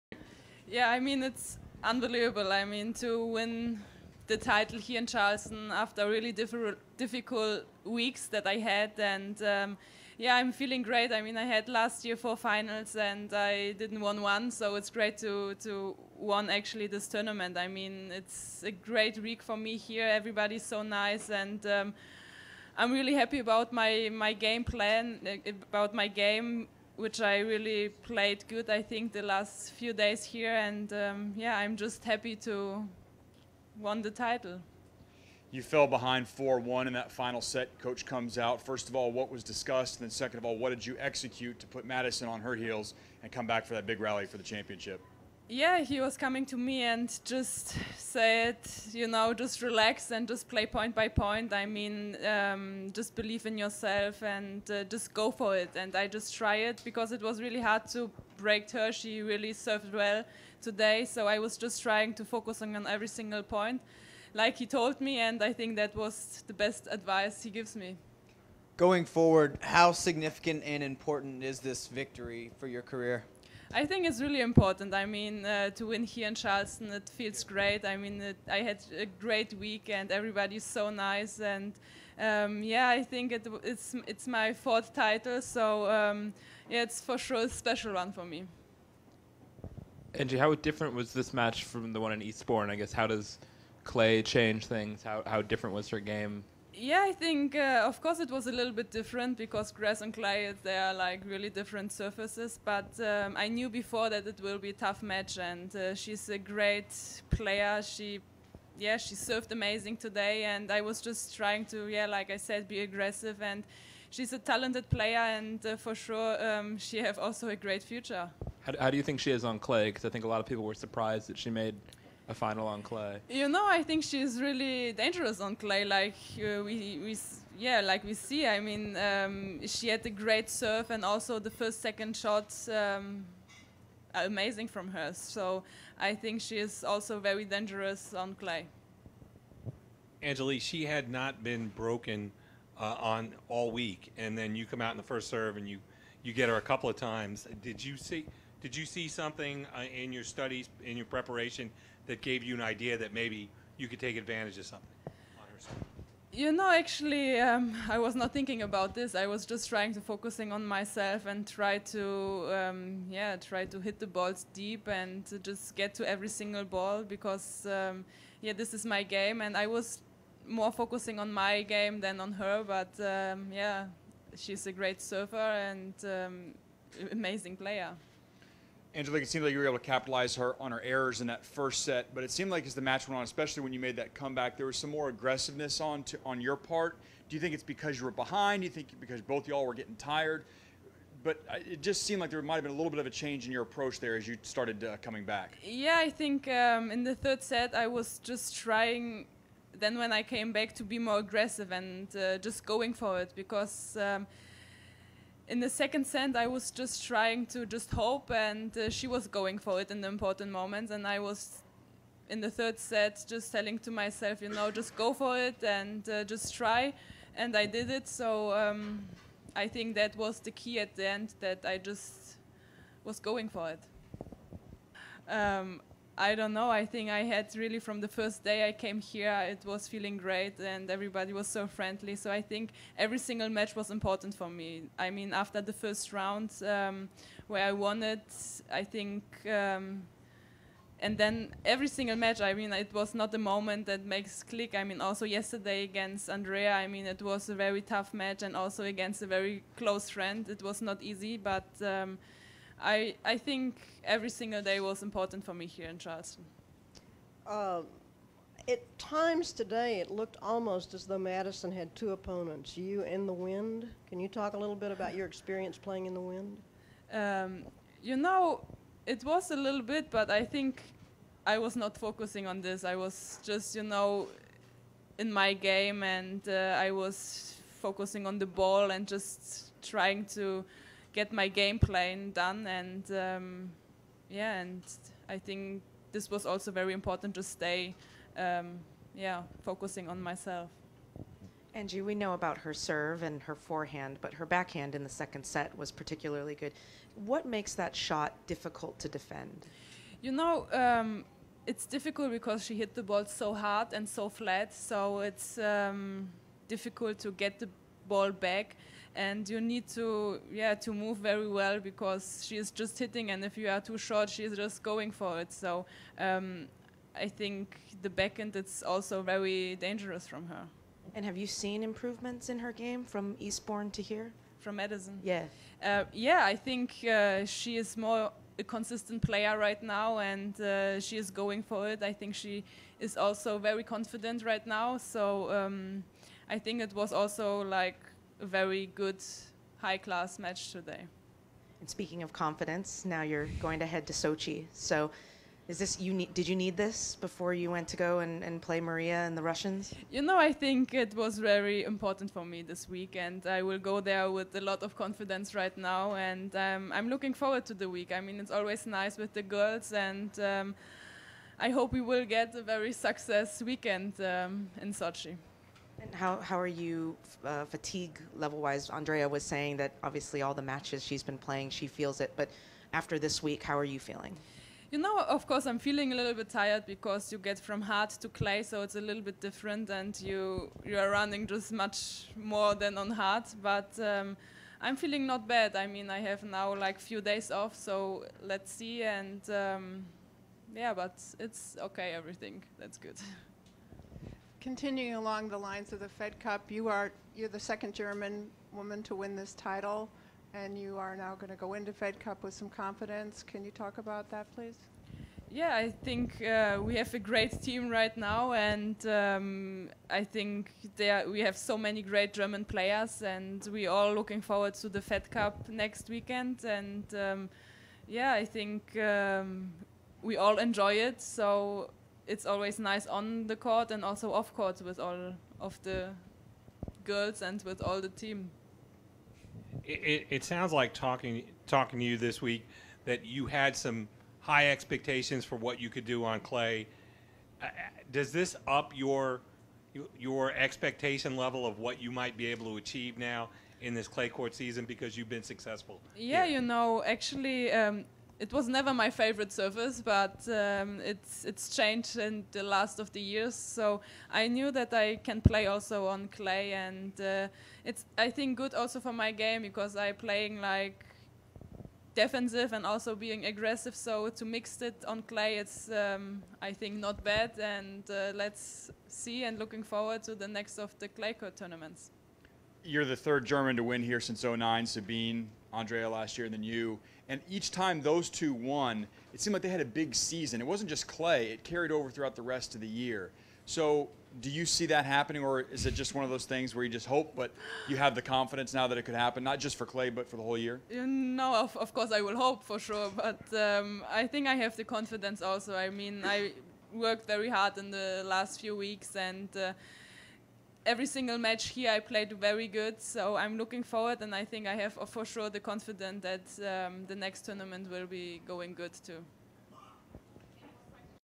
RAW: Angelique Kerber After Winning The Family Circle Cup
Angelique Kerber's Interview After Winning The Family Circle Cup